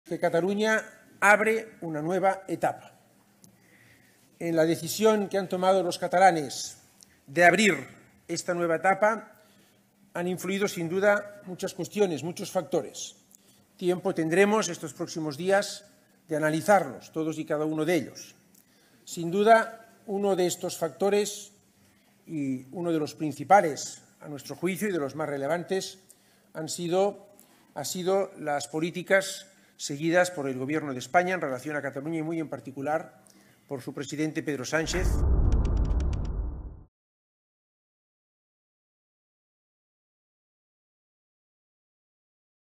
Illa vence con holgura e intentará gobernar para "abrir una nueva etapa en Cataluña"
El personalista pulso entre Pedro Sánchez y Carles Puigdemont, los padres de la amnistía, se saldó con una cómoda victoria del socialismo en Cataluña; con el resultado que el presidente del Gobierno debió estar calculando durante sus cinco días de reflexión en La Moncloa. El PSC no sólo ganó las elecciones, sino que lo hizo con siete escaños de ventaja sobre Junts, evaporando el espíritu de remontada que el prófugo se encargó de alimentar durante la última semana de campaña para alentar un sorpasso que estuvo lejos de producirse.